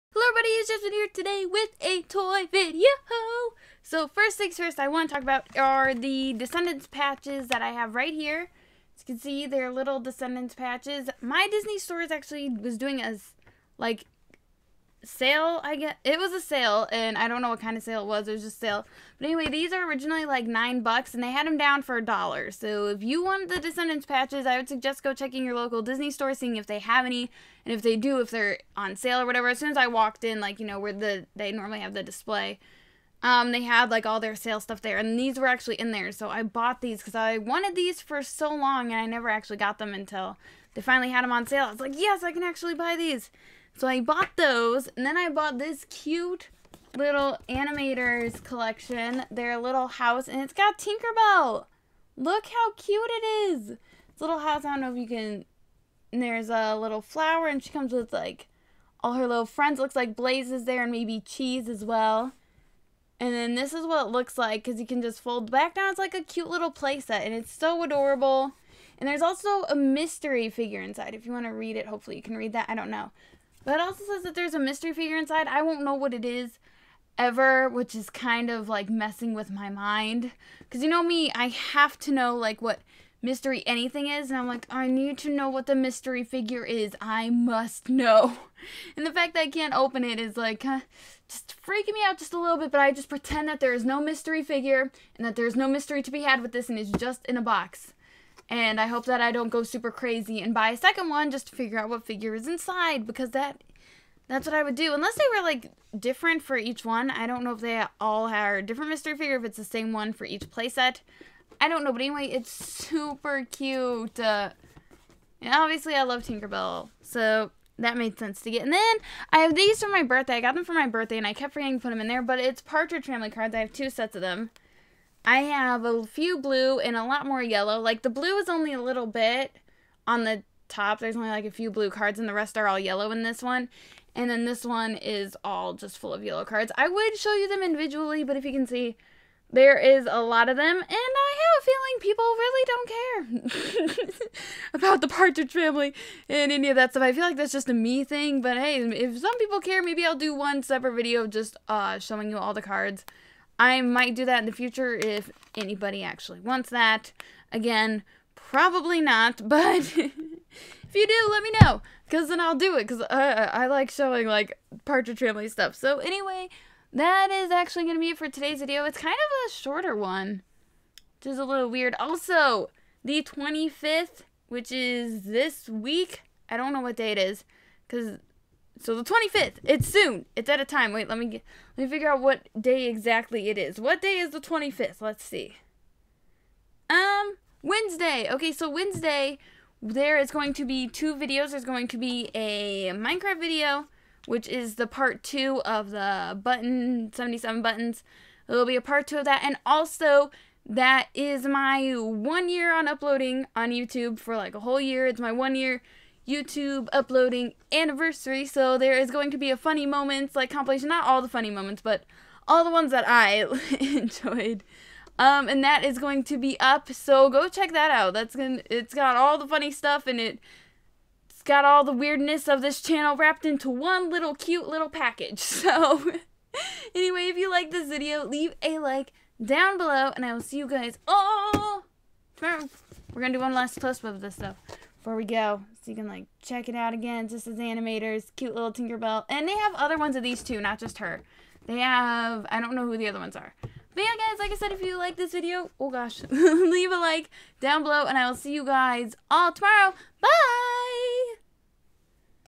Tinker Bell Disney Animator's Collection Littles / Descendants Patches - Toy Haul Ep 33
Today I show you the new Disney animator's collection littles Tinker Bell and Descendants Patches and the Partridge family cards I got for my birthday Enjoy!!!